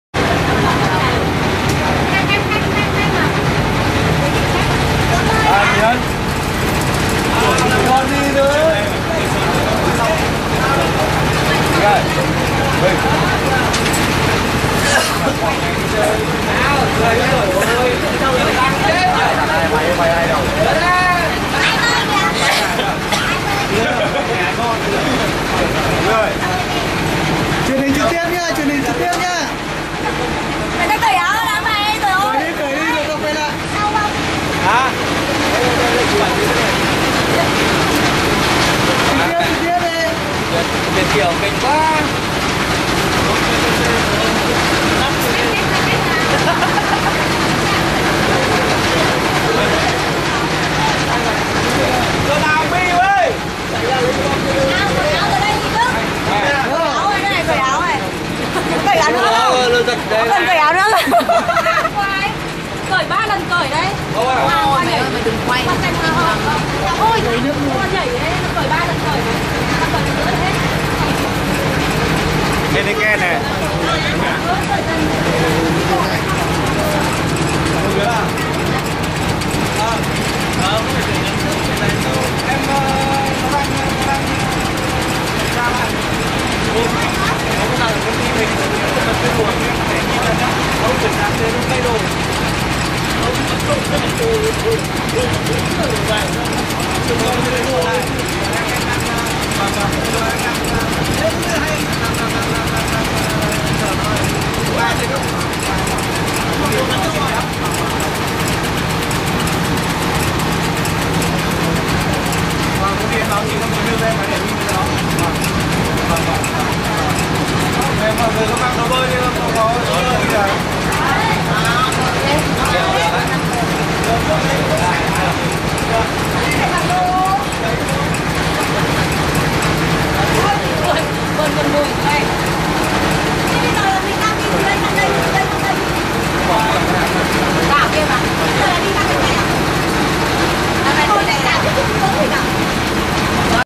12C-Hai Bà Trưng- họp lớp 2010
Lop 12C truong Hai Ba Trung, Hanoi, nien khóa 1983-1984 tổ chức họp lơp đi chơi hồ Đại Lải ngày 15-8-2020